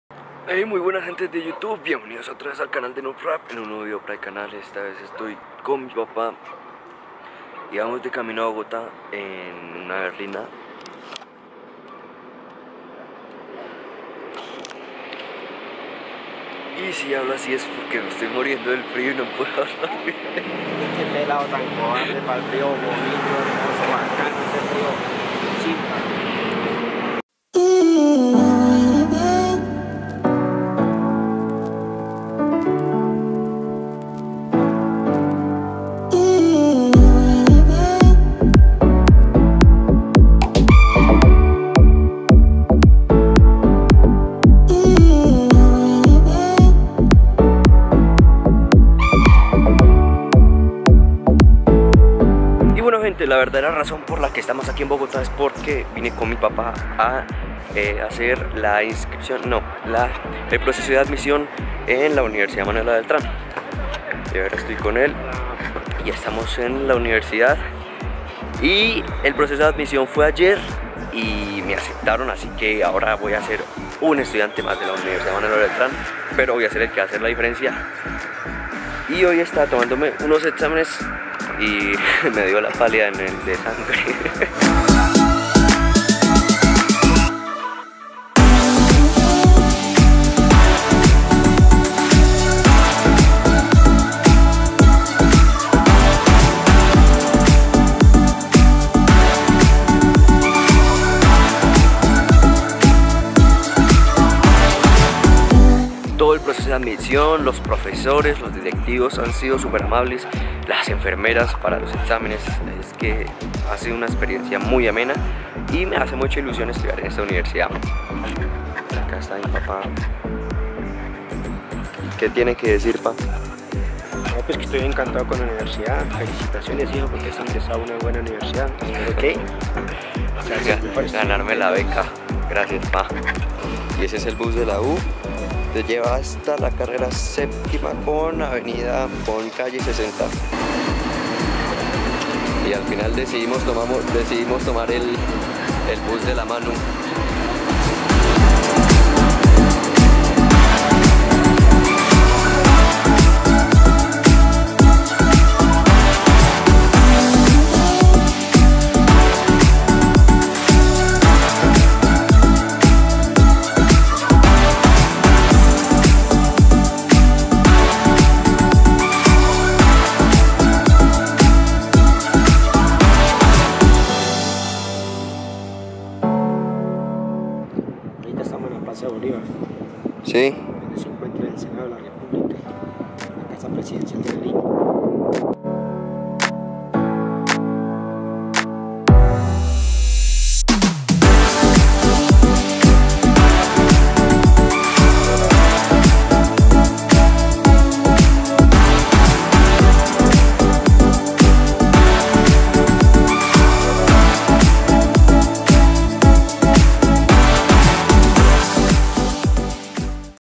VLOG: BOGOTÁ Y LA MANUELA BELTRÁN
Muy buenas! Bienvenidos otra vez a NoobRap. Esta vez con un nuevo vídeo junto a mi padre de mi última ida a Bogotá. ¡Espero lo disfruten!